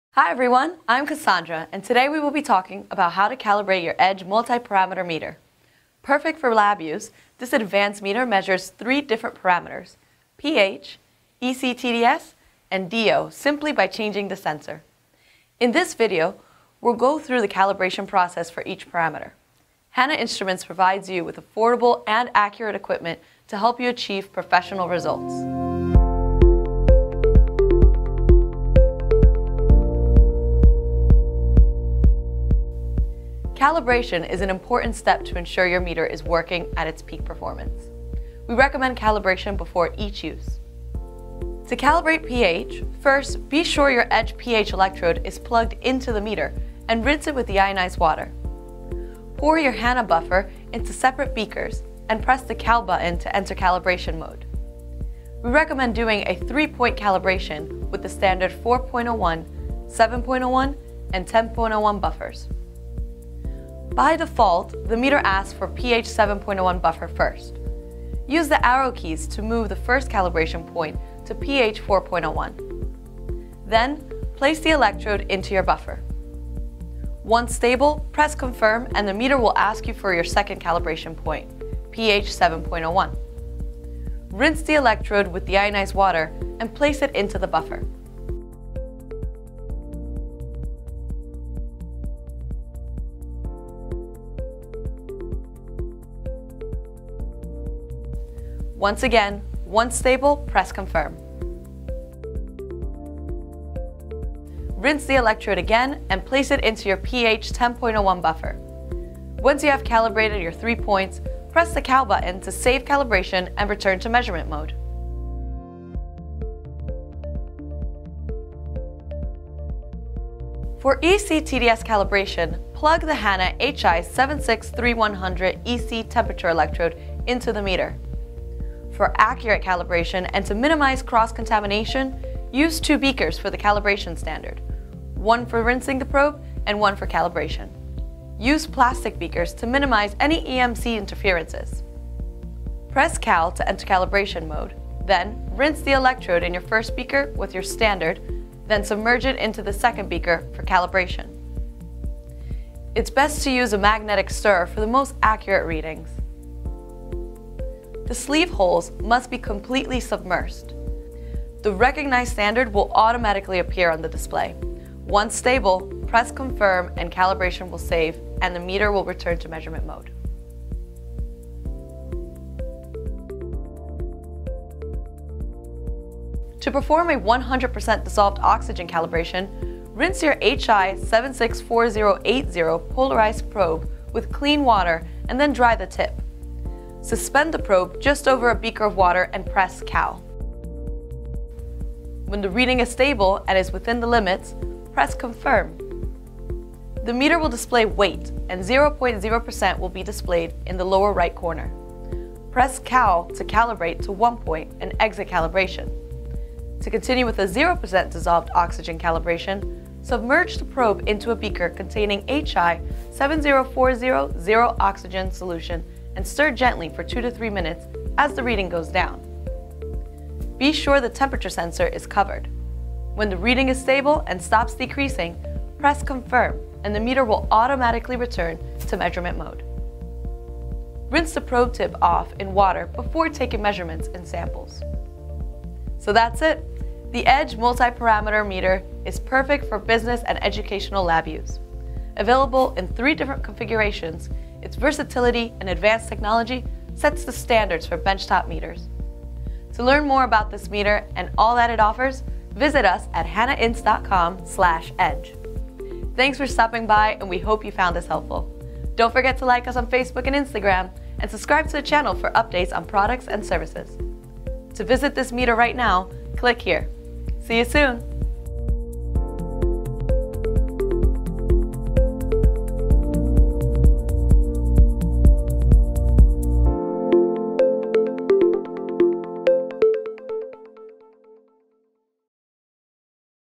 Cómo calibrar el medidor de pH multiparámetro edge® de Hanna Instruments®
Aprenda a calibrar nuestro medidor de pH multiparámetro edge®.